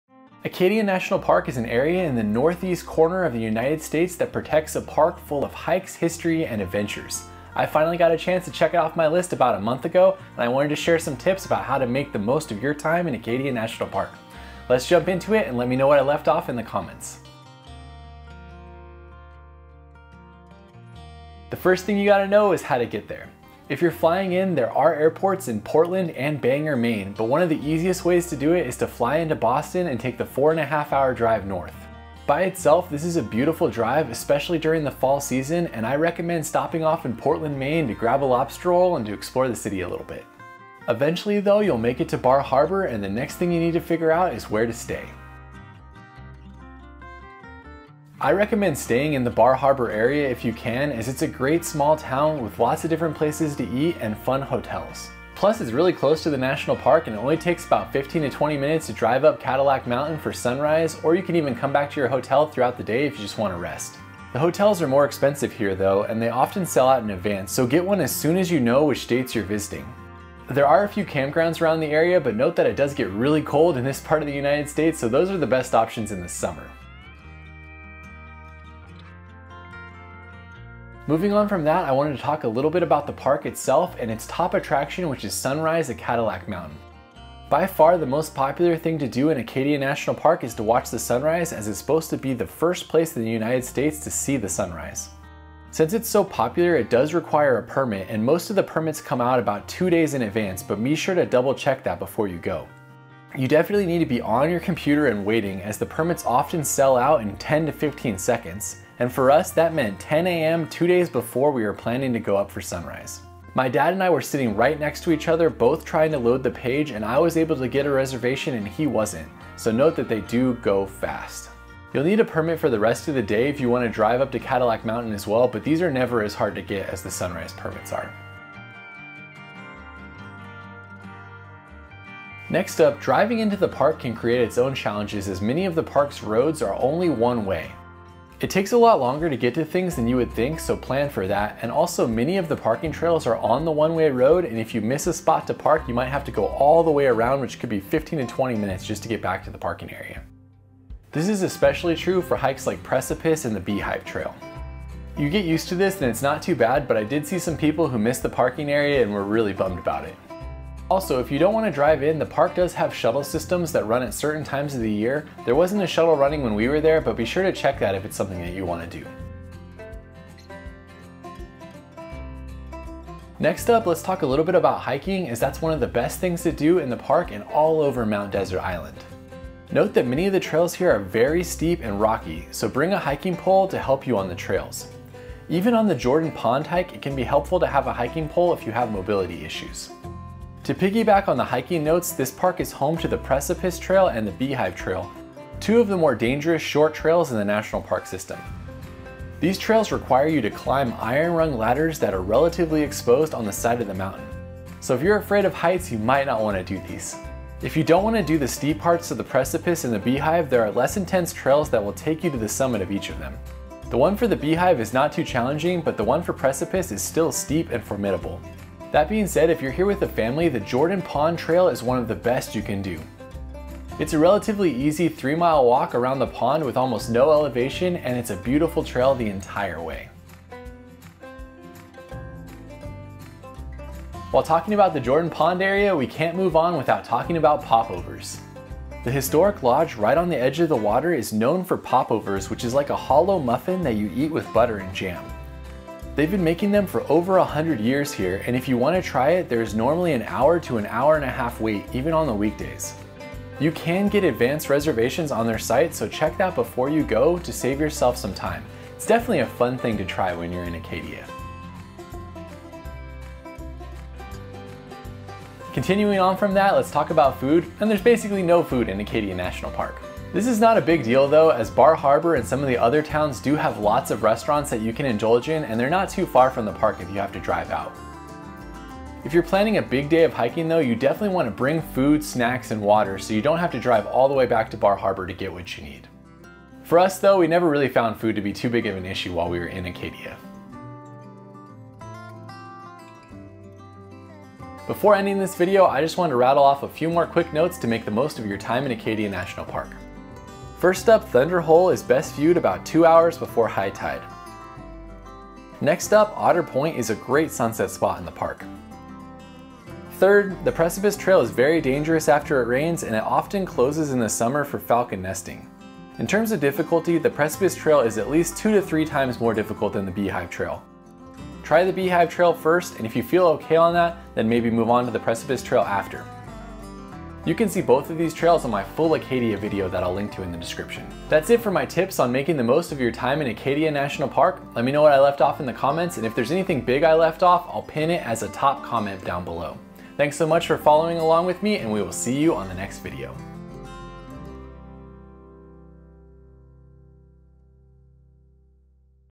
Acadia National Park Tips for First Time Visitors
Acadia National Park is one of the most visited national parks in the United States. It is a beautiful area to explore with amazing hiking trails, history and views. Here is how you can make the most of your time in Acadia National Park as a first time visitor. Let me know if you have more tips in the comments.

Cadillac Summit Road Sunrise Reservations (needed from May 24 - Oct 22)

Chapters
0:00 Intro
0:19 How do you get to Acadia?
0:46 Where should I stay?
1:19 Sunrise permits for Cadillac Mountain
2:10 How to get around the park
2:50 Hiking in Acadia National Park
3:51 Jordon Pond Reservations
4:22 Food in Acadia National Park
4:52 Three More Quick Tips